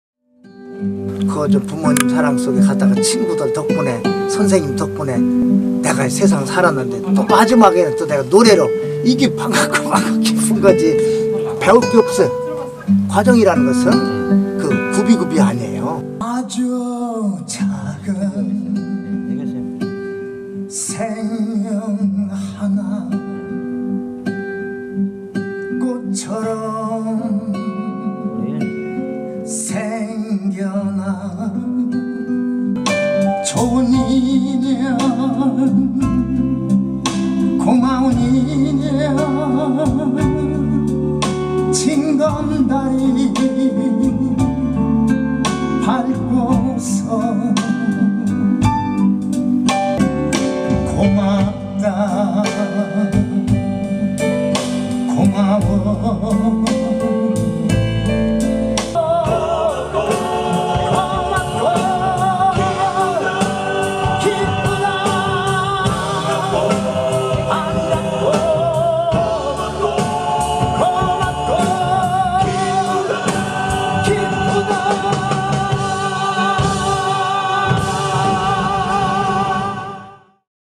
장사익 20주년 '반갑고 고맙고 기쁘다'
장사익은 곧 데뷔 20주년을 맞아 공연을 펼친다. 앨범도 함께 준비하고 있다. “10년 전 공연에서 ‘10년 뒤 다시 만나자’ 했던 약속을 지키게 돼 기쁘다”고 말했다. /이태경 기자

가늘고 길게 가야 돼 우덜은, 삐걱거리면서